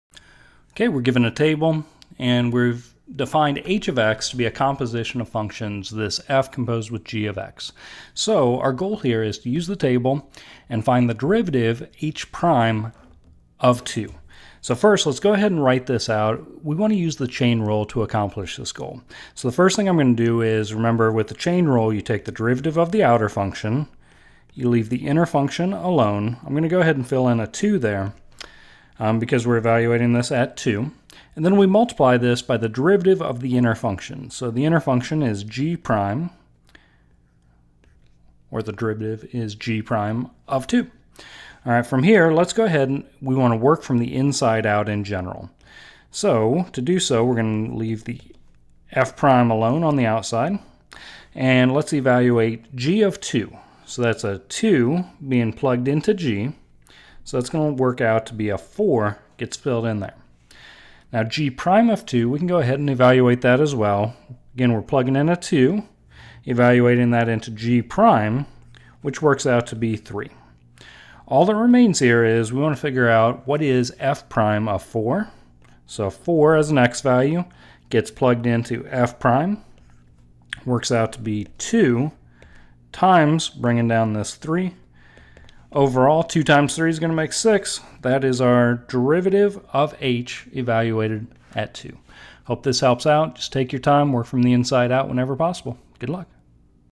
Chain Rule | Table
In this example problem, we write out the chain rule to find the derivative of our function because it is defined to be the composition of functions.  We then use the provided table to find the derivative.